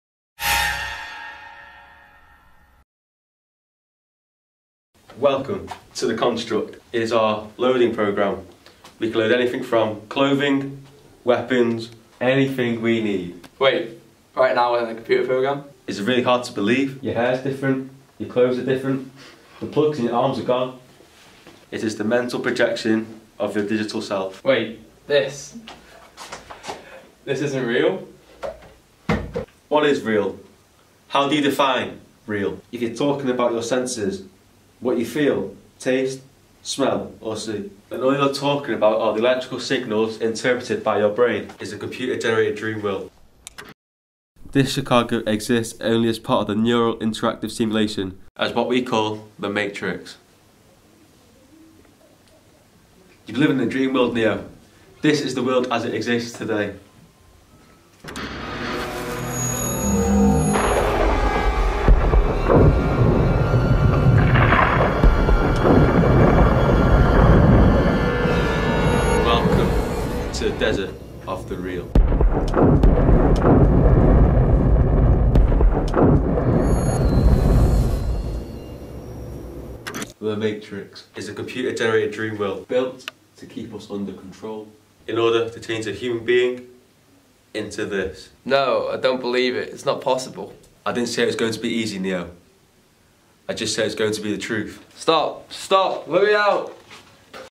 Matrix construct scene recreation
Recreation of  the construct scene out of Matrix for media